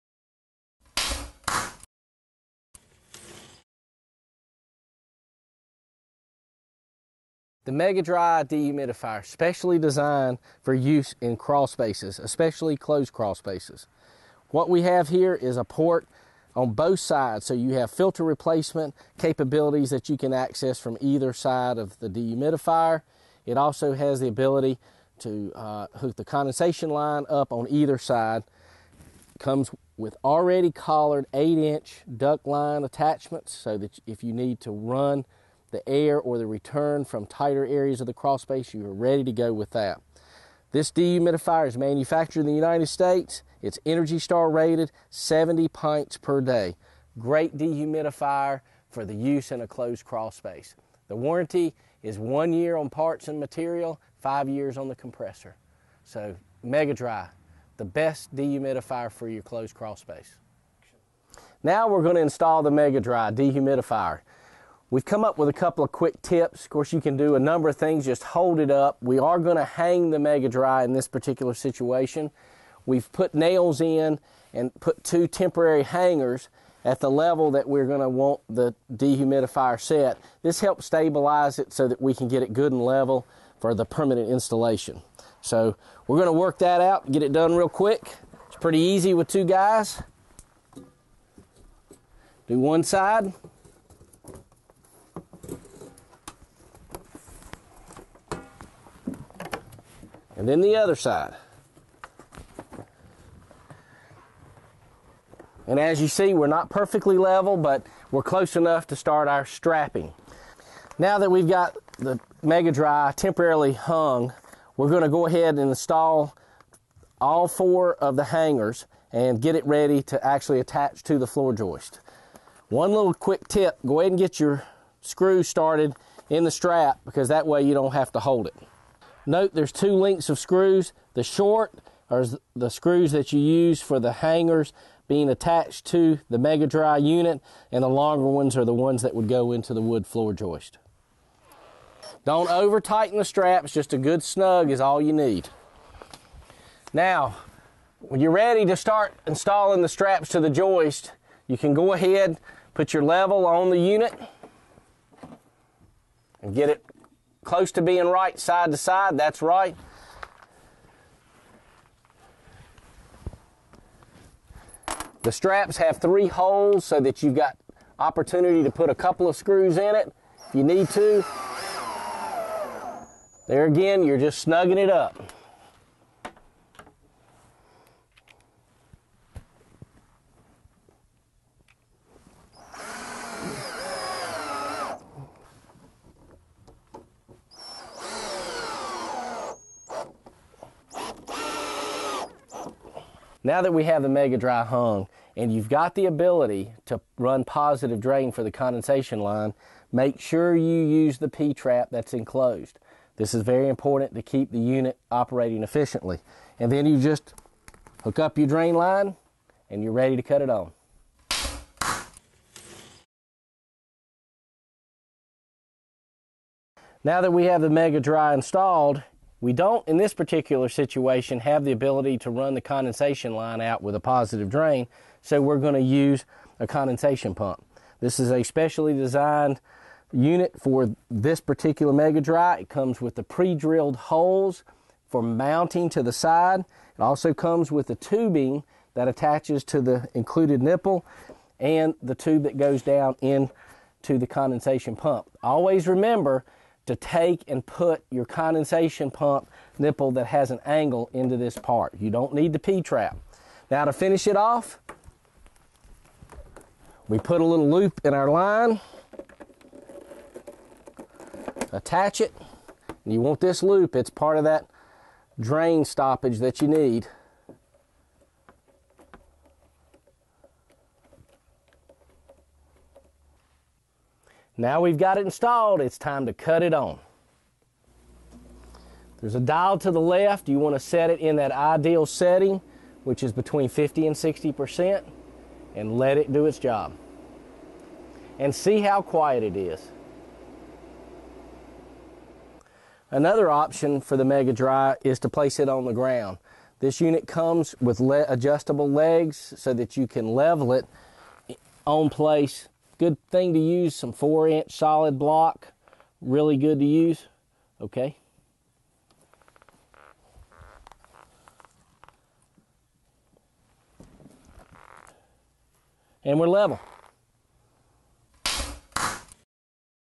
Mega Dry Dehumidifier from Crawlspace Depot
This Mega Dry Crawlspace Depot video includes product features as well as directions for installing the dehumidifier and the optional condensate pump. For more information and to purchase closed crawlspace products,